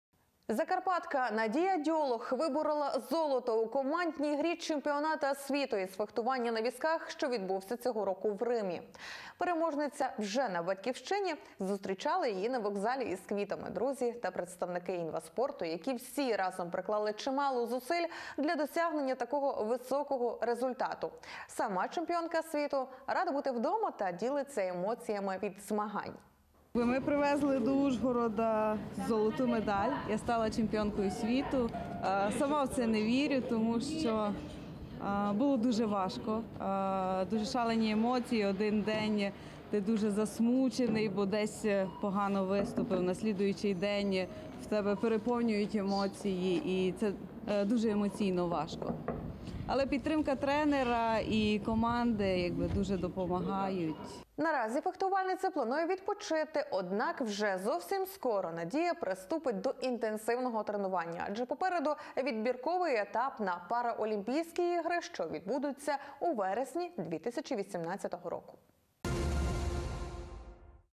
Закарпатка виборола золото на Чемпіонаті світу з фехтування на візках
Закарпатка Надія Дьолог виборола золото в командній грі Чемпіонату світу з фехтування на візках, що відбувся цього року в Римі. Переможниця вже на Батьківщині, зустрічали її на вокзалі з квітами друзі та представники Інваспорту, які всі разом приклали чимало зусиль для досягнення такого високого результату. Сама чемпіонка світу рада бути вдома та ділиться емоціями від змагань.
Наразі фехтувальниця планує відпочити, однак вже зовсім скоро Надія приступить до інтенсивного тренування, адже попереду відбірковий етап на параолімпійські ігри, що відбудеться у вересні 2018 року.